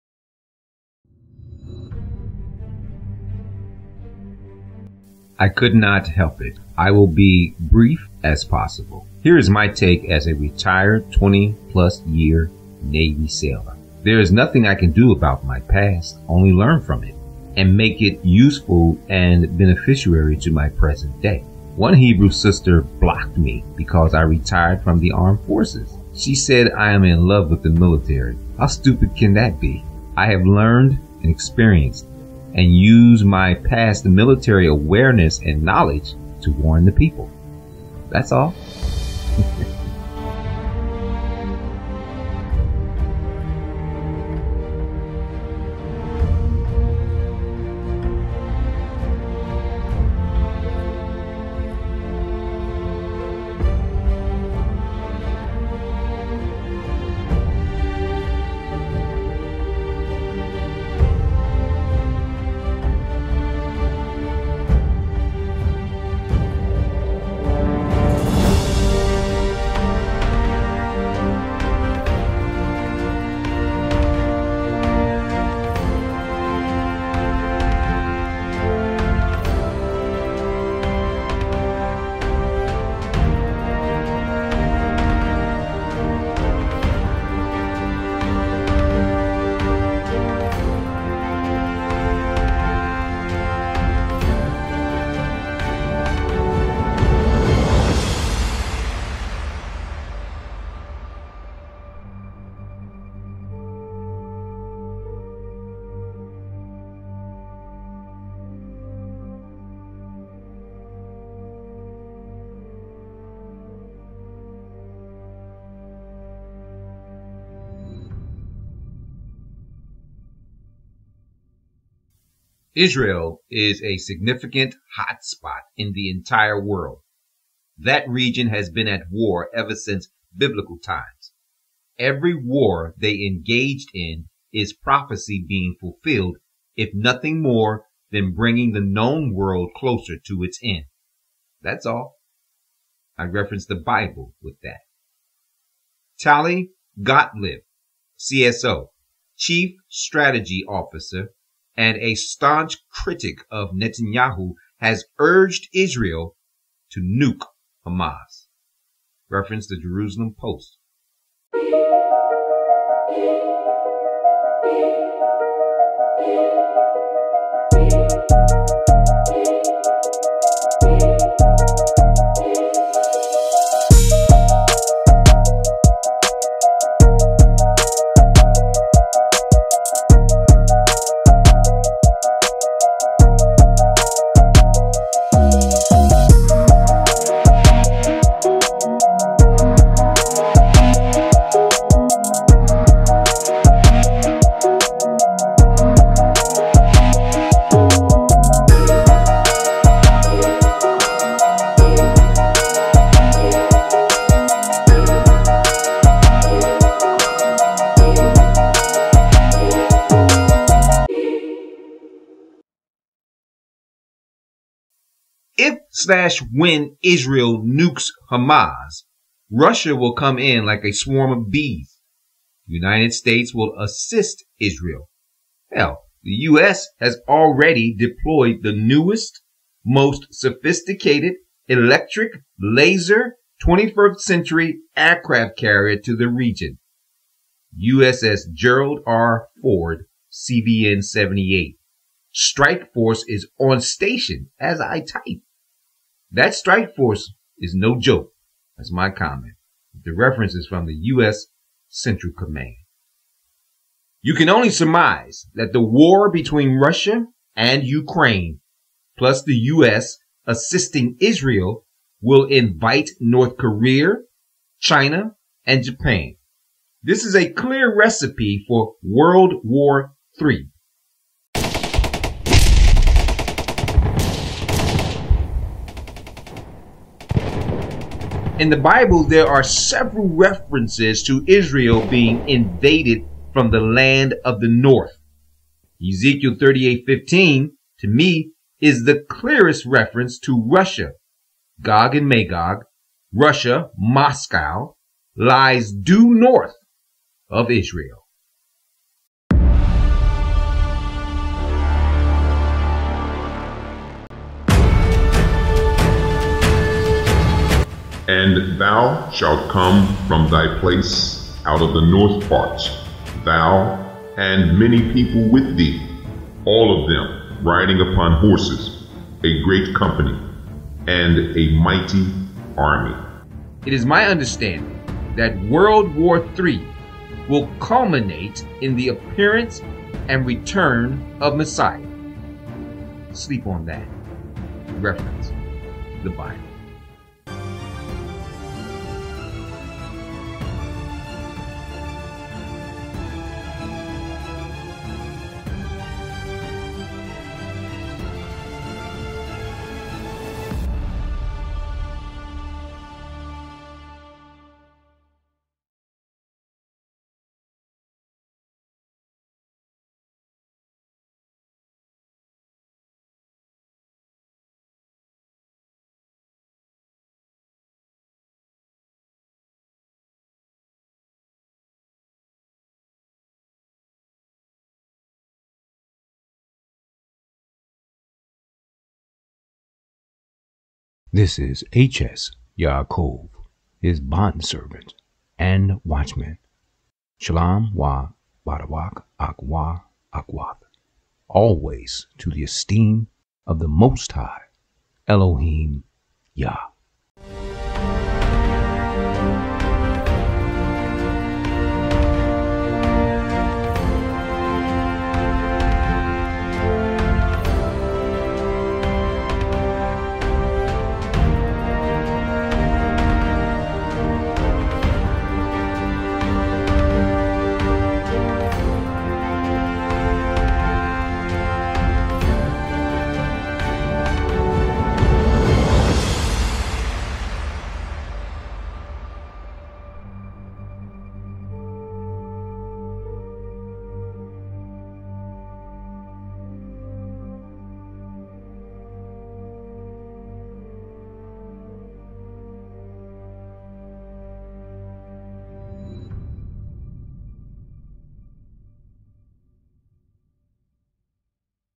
Israel vs Hamas
A Biblical scenario of the end of the war between Russia and Ukrain.